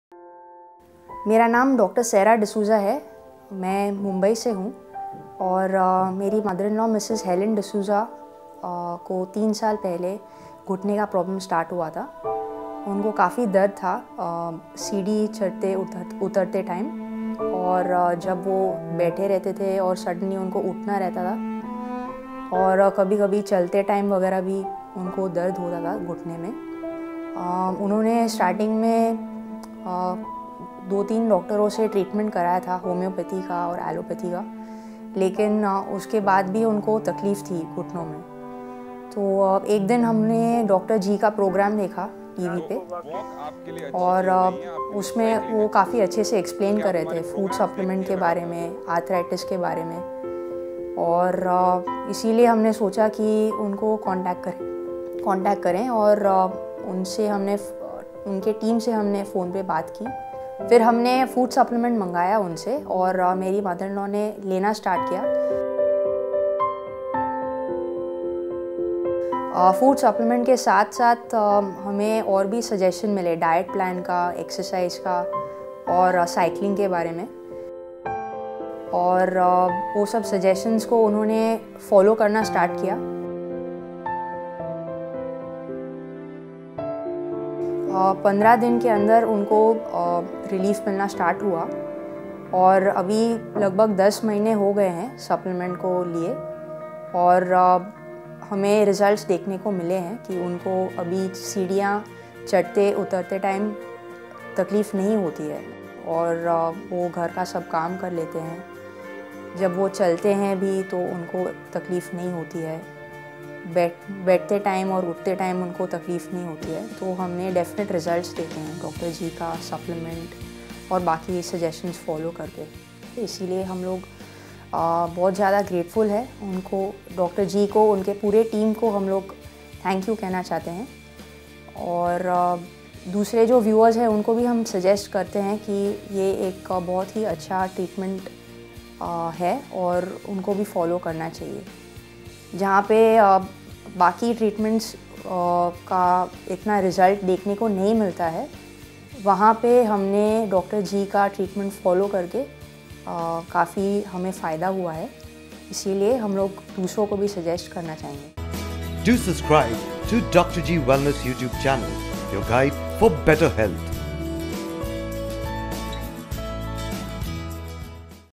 Mrs. Helen D'Souza Success Story - Knee Pain Relief - Dr. Gaurav Sharma
"Mrs. Helen D'Souza an elderly lady overcame knee pain after being treated under Dr. Gaurav Sharma's treatment.
Reversing Arthritis is a presentation where renowned lifestyle doctor and sports medicine expert Dr. Gaurav Sharma, popularly known as Dr. G guides us with his scientific ways to reverse the arthritis & osteoarthritic pain and its effects."